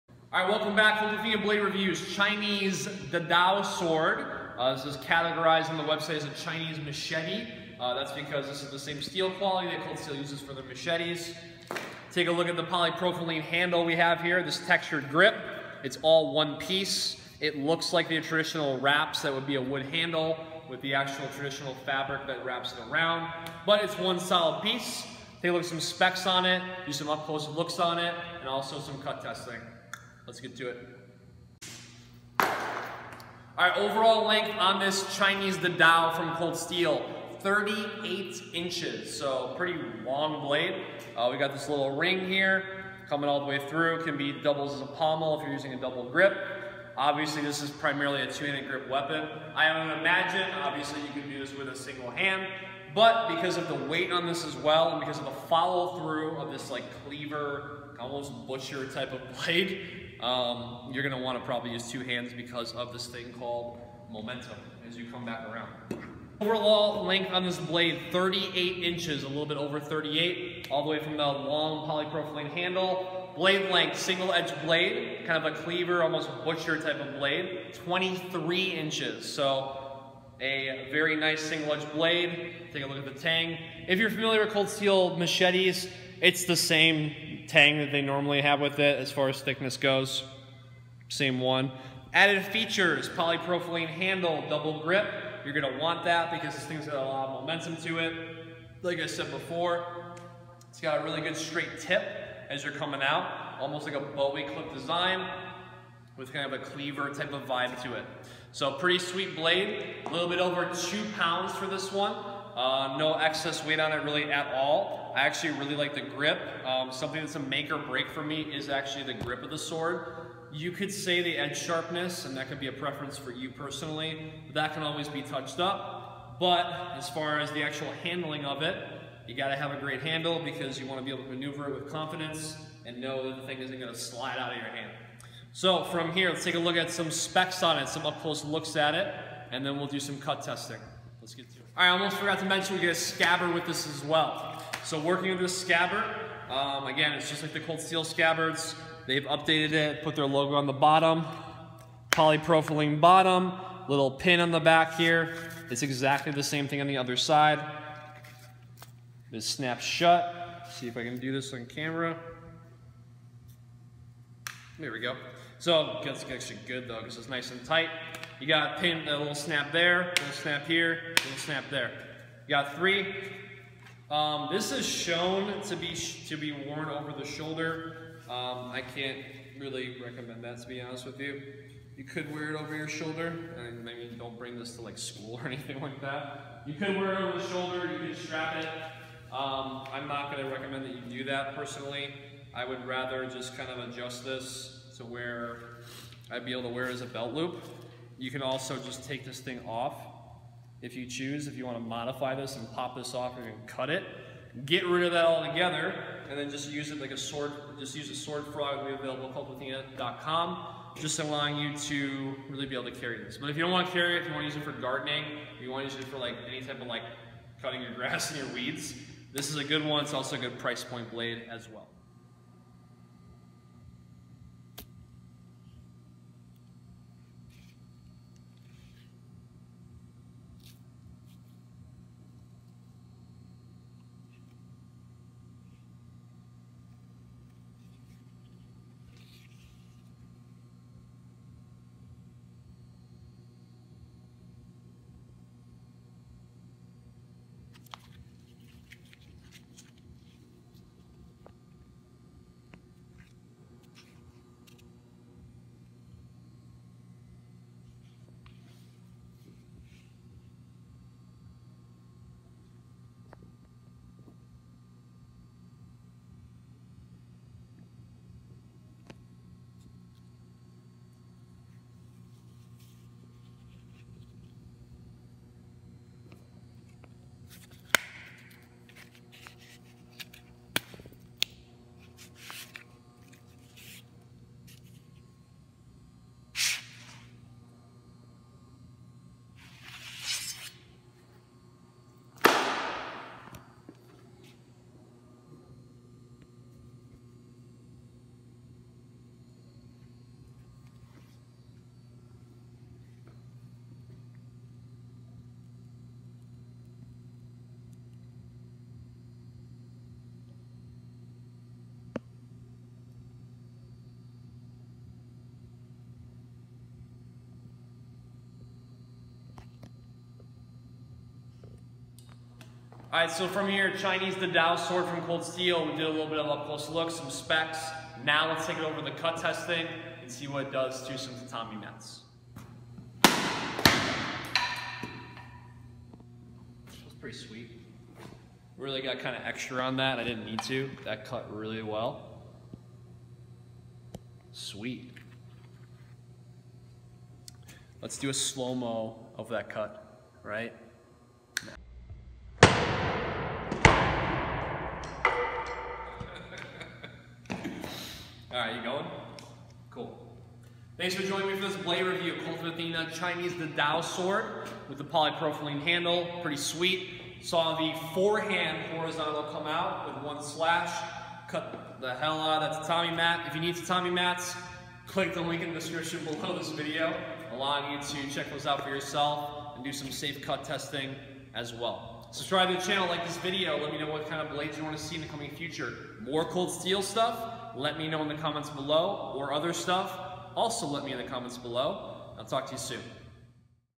Cold Steel Chinese Sword Machete | Review | Kult of Athena
Cold Steels Dadao Machete is an excellent way to harness the ferocious cleaving power of the Chinese Dadao at a budget price; the blade is tempered 1055 high carbon steel and it possesses a sharp edge and a black, baked-on rust-resistance finish. The full tang is fitted with a tough polypropylene handle with molded cord detailing for an improved grip. The sword comes with a nylon Cor-Ex sheath with buttoned retaining straps and a hard impact plastic tip. It has a long, adjustable strap which allows it to be worn on the back.